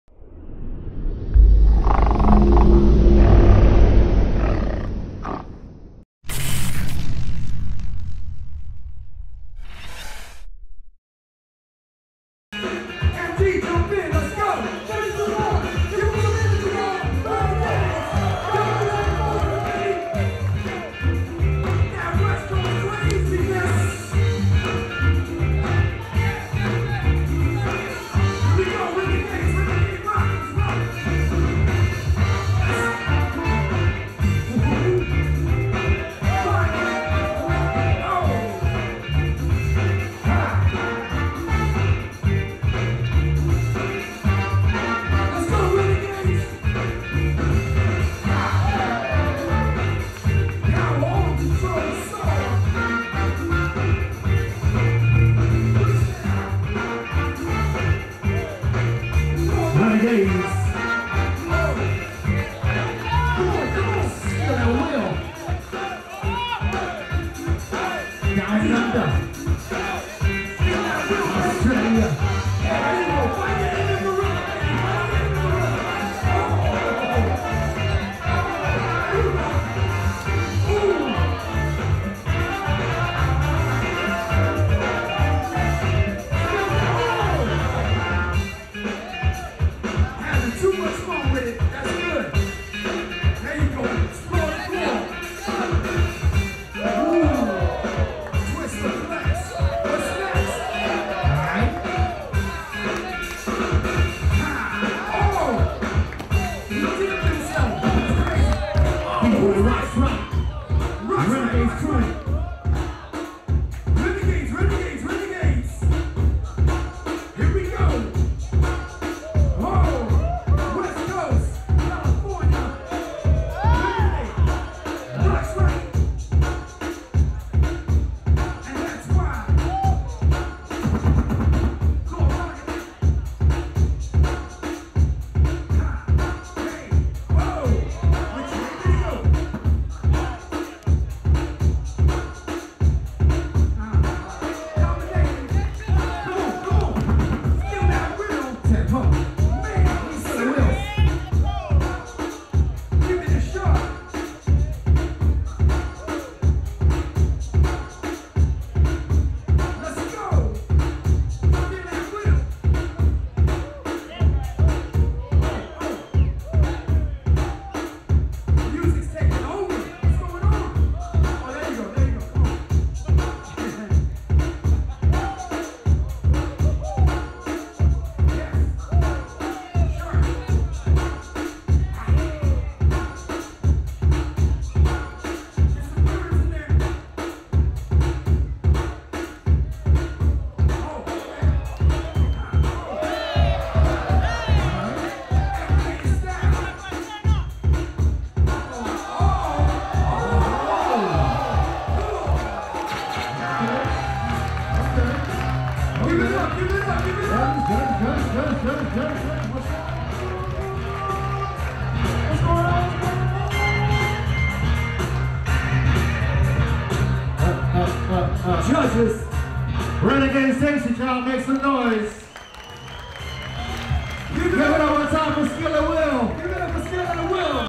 Skill @ Will Vs Renegades - Top 32 - Freestyle Session 2018 - Pro Breaking Tour
Freestyle Session World Finals 2018 (21)
Powered by Monster Energy & Tiger Balm Active

2 v 2 Top 32

Winners
Renegades

Judges
Storm
Orko
Jeromeskee
Skim
Wicket

Alternate/Prelim judges
Renegade
Jeskilz
Goku(Charles Soul Control)
Eddie Styles

DJ
Element
Timber

Emcee
Cros1
Ivan The Urban Action Figure